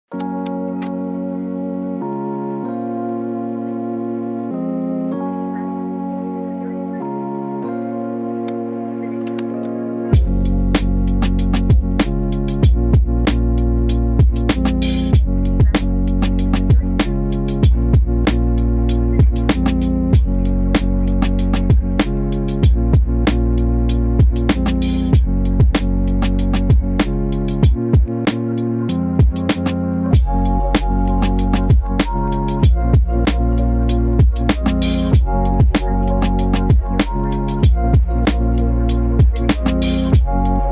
Sony a6700 UNDERWATER down to 200 FEET!
Take the Sony a6700 down to 200 feet in the Ikelite Underwater Housing!